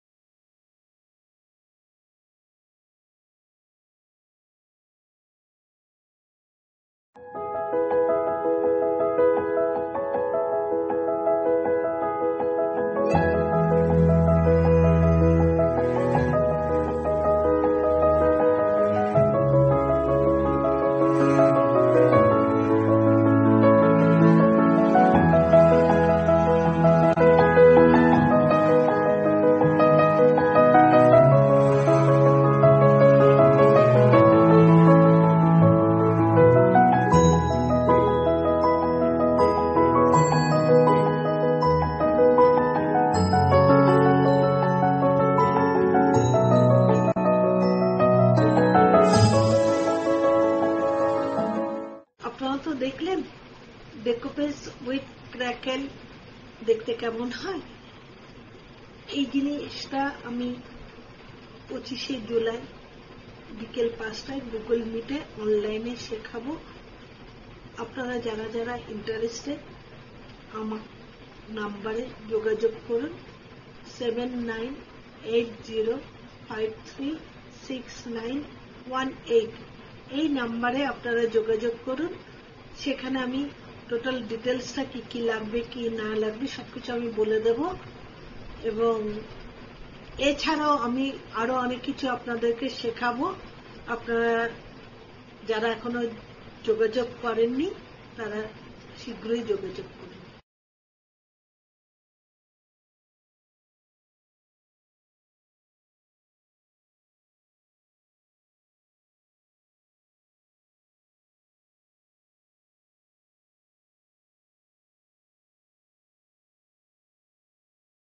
Learn Decoupage on crackle effects online on 25th July 5pm | Art. Shilpi Kundu
Eminent artist Shilpi Kundu is arranging an art and craft workshop over Google MEET  on the exciting art form- DECOUPAGE ON CRACKLE EFFECTS.  It is made mostly of simple household items. And the best part is you do not have to pay anything to learn this technique! the workshop will be conducted on 25th July, from 5 pm onwards.